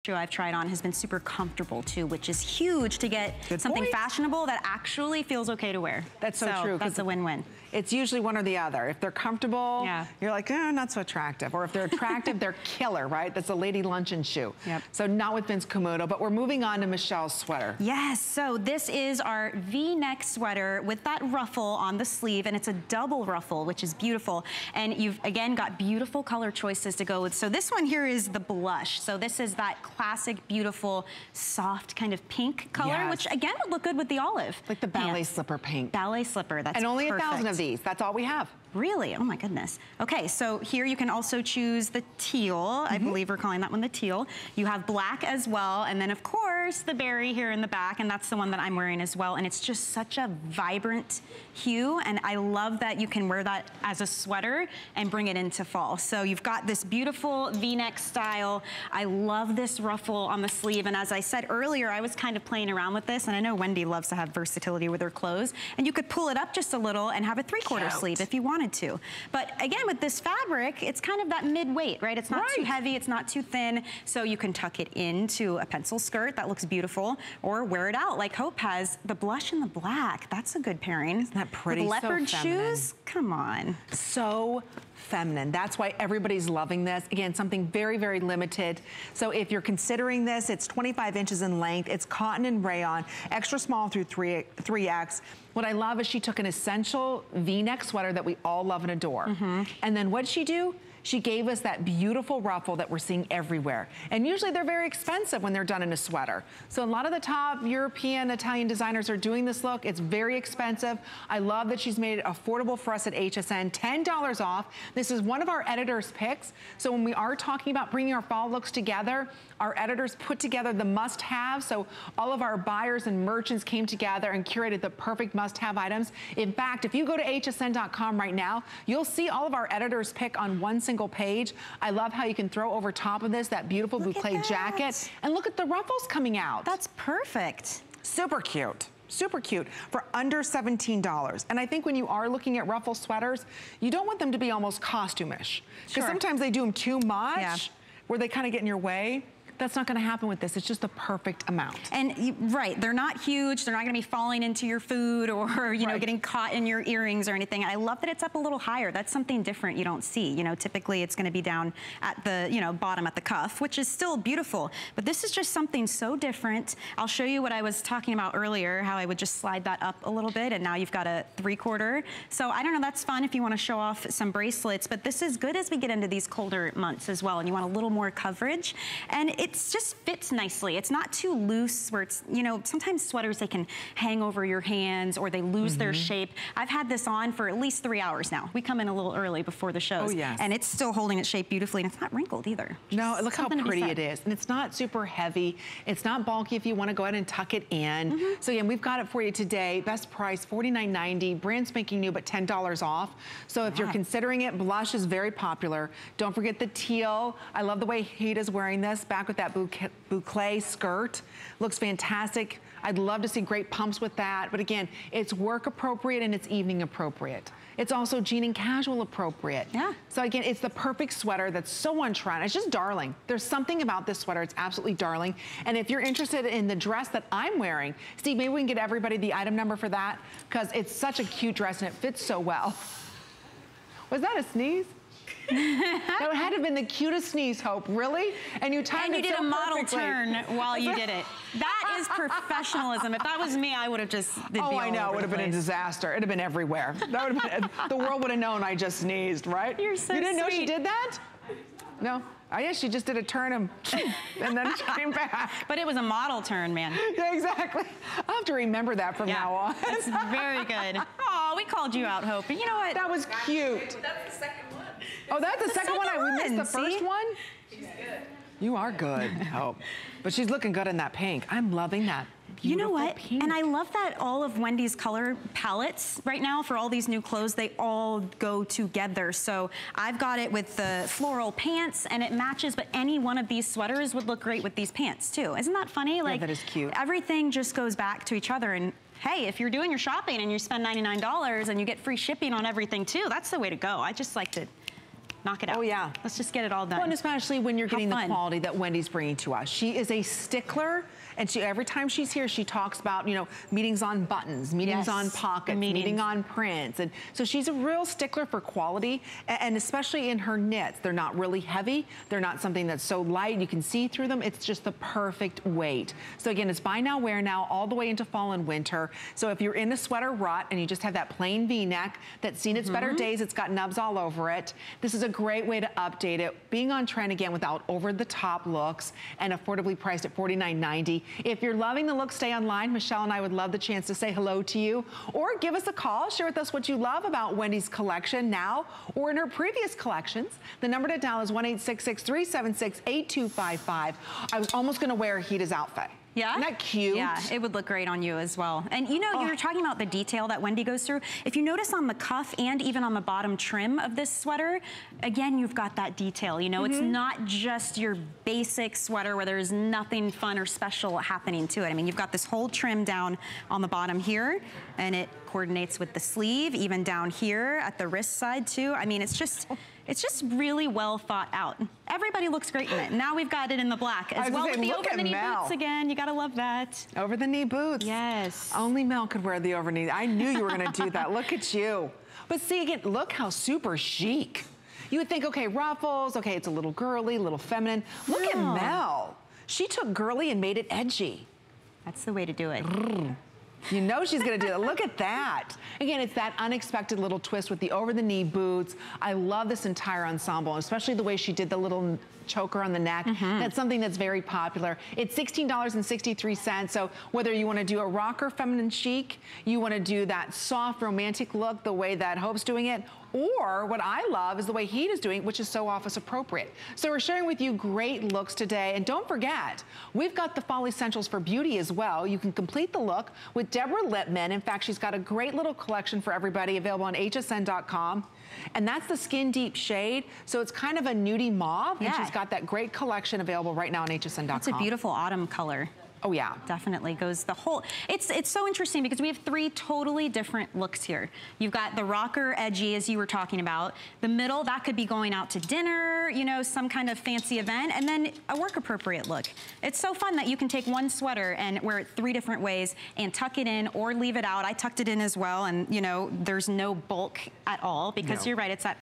Wendy Williams VNeck Sweater with Ruffle Sleeve
Put a little chacha into your everyday dressing with a soft, sassy sweater that's ready to move! Double ruffle sleeves and a classic sweater profile...